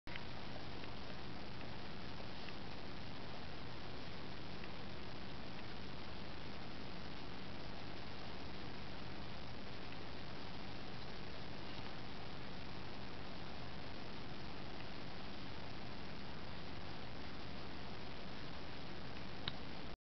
Maddy Boomer Titan Snuggle
One of our dogs, one of our cats and one of the neighbors kitten's napping on our couch.